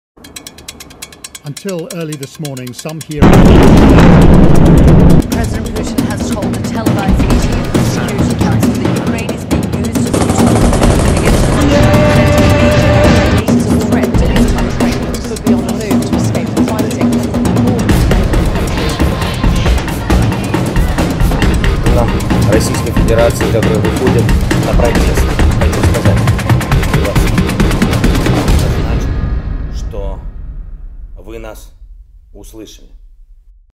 Мы сражаемся за НАШУ СВОБОДУ в своей стране – Украине
Вам говорят: "Городам ничего не угрожает", но это не наши танки на улицах бомбят наши дома – они ваши! Мы вам говорим, что стоим на своей земле и сражаемся за свою свободу в СВОЕЙ стране – Украине. Мы сражаемся за жизнь наших детей и нам не нужно ваше "спасение". Убирайтесь с нашей земли. Хотите помочь? выходите на митинги за нашу и вашу свободу. Не бойтесь, вы не капля в океане. Вы уже целый океан, В КОТОРОМ ПУТИН ЗАХЛЕБНЁТСЯ.